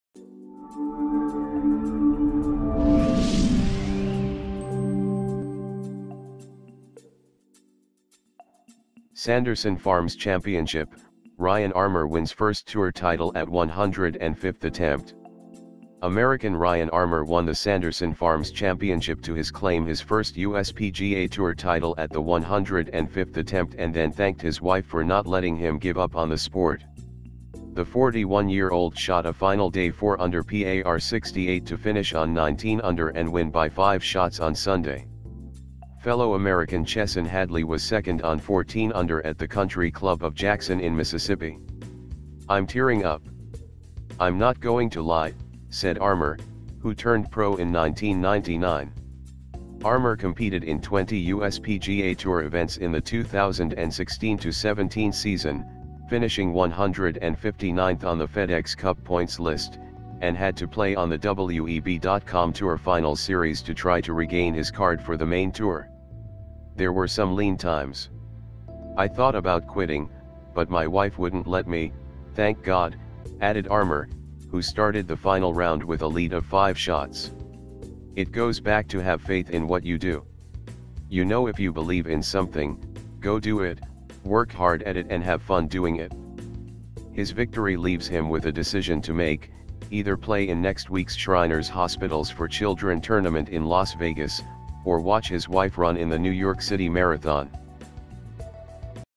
AMAZING sanderson farms championship
Sanderson Farms Championship: Ryan Armour wins first Tour title at 105th attempt
American Ryan Armour won the Sanderson Farms Championship to his claim his first US PGA Tour title at the 105th attempt and then thanked his wife for not letting him give up on the sport.
The 41-year-old shot a final day four-under-par 68 to finish on 19 under and win by five shots on Sunday.
Fellow American Chesson Hadley was second on 14 under at the Country Club of Jackson in Mississippi.
"I'm tearing up. I'm not going to lie," said Armour, who turned pro in 1999.